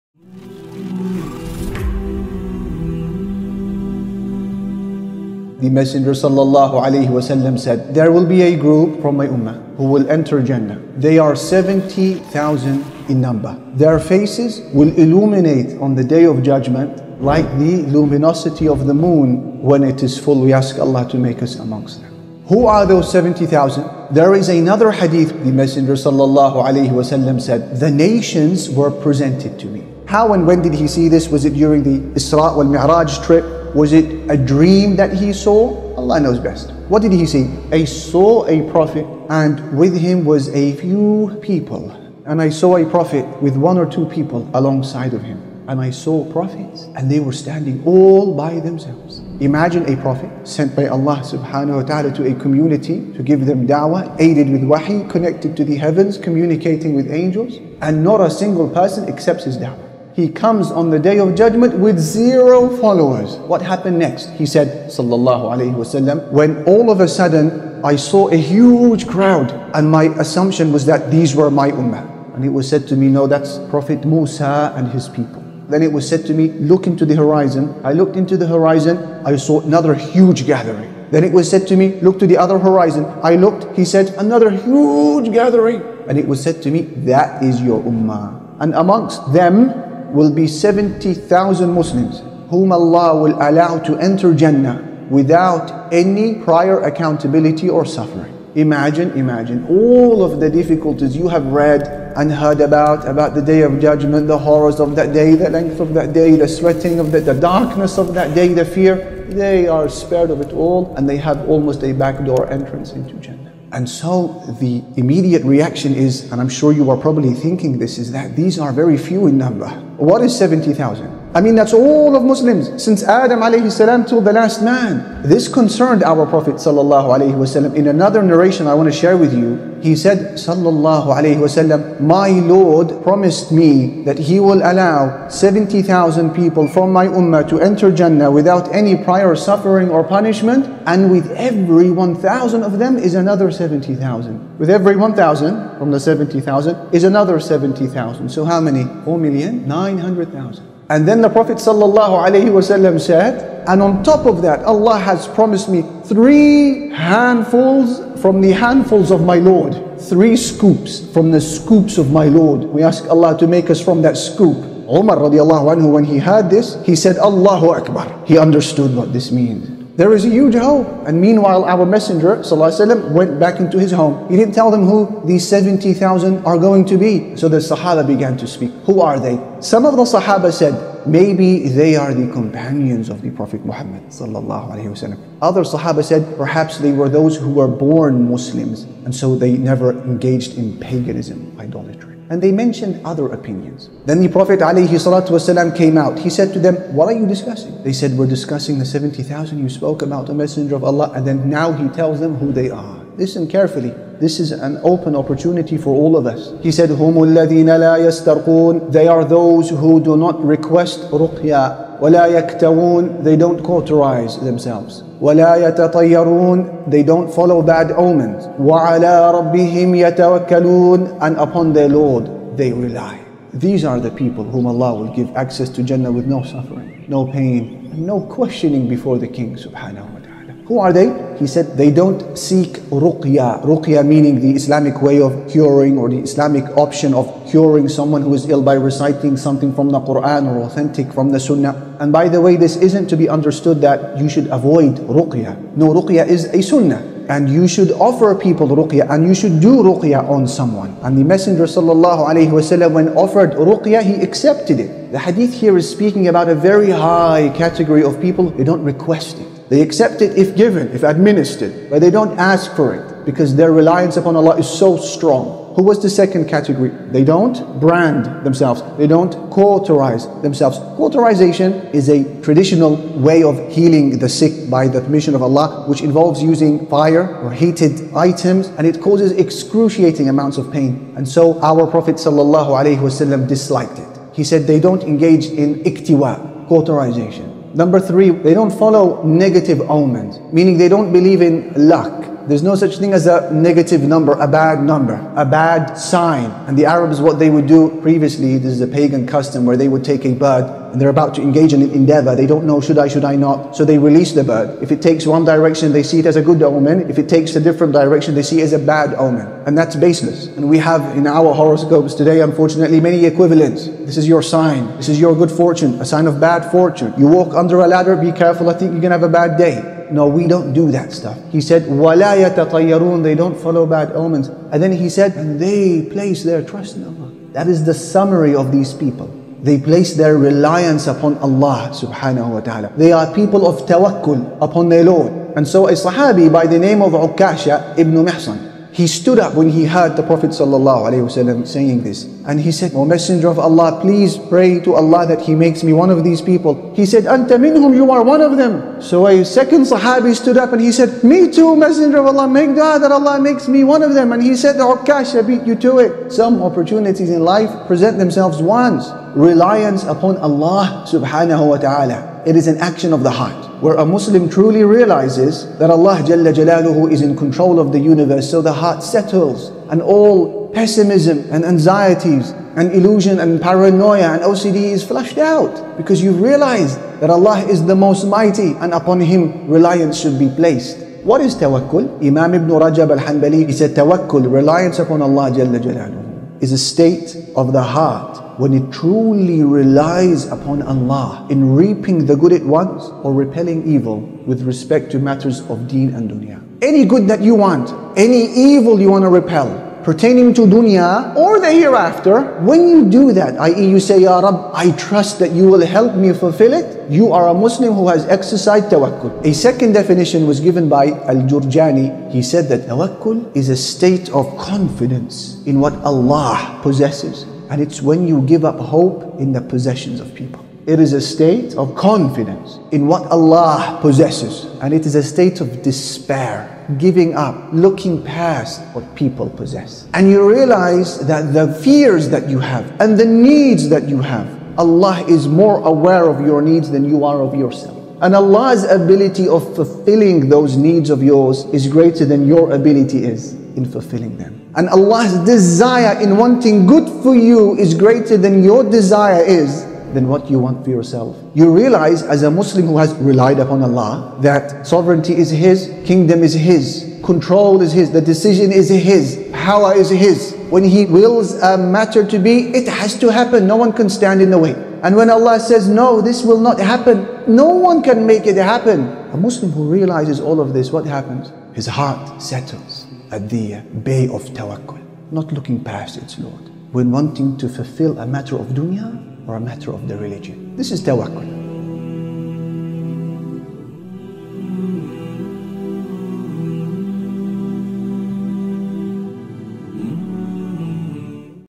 4 QUALITIES OF 70,000 MUSLIMS MUHAMMAD (ﷺ) PREDICTED!
Feed Gaza's Children! - $96 Feeds A Child For 6 Months! Donate @

4 QUALITIES OF 70,000 MUSLIMS MUHAMMAD (ﷺ) PREDICTED!

Speaker: Ustadh Ali Hammuda